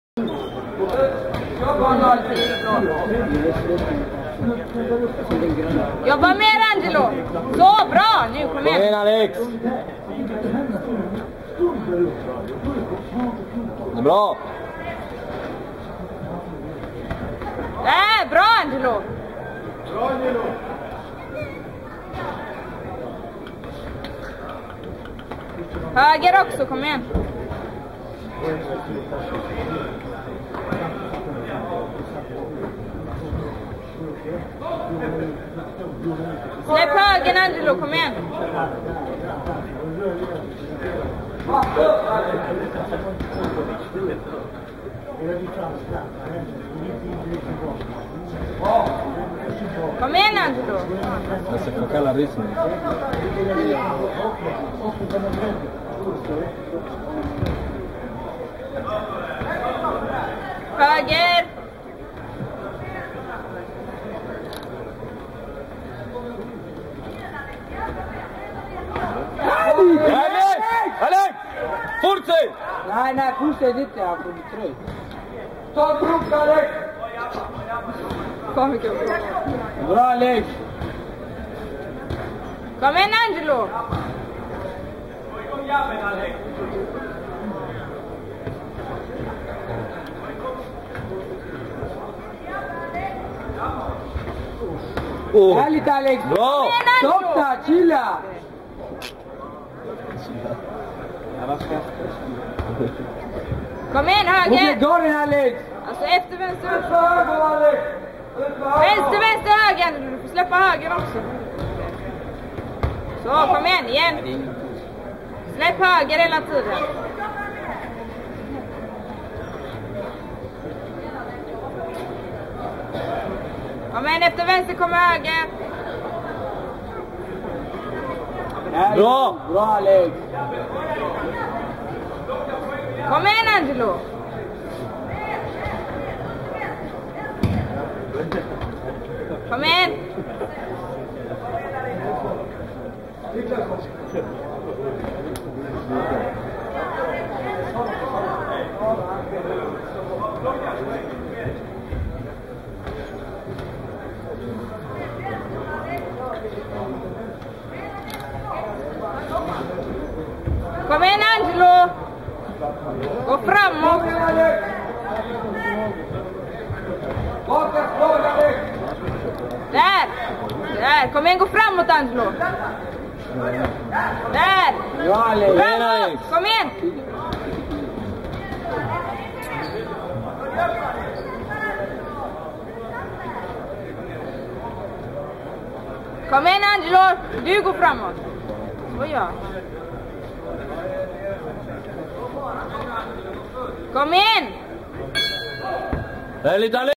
Angelo Lundgren vs Alexander Potter semifinal rond 2 SM i boxning 2009
Angelo Lundgren från Angered BC vs Alexander Potterfrån Winning BC semifinal rond 2 SM i boxning 2009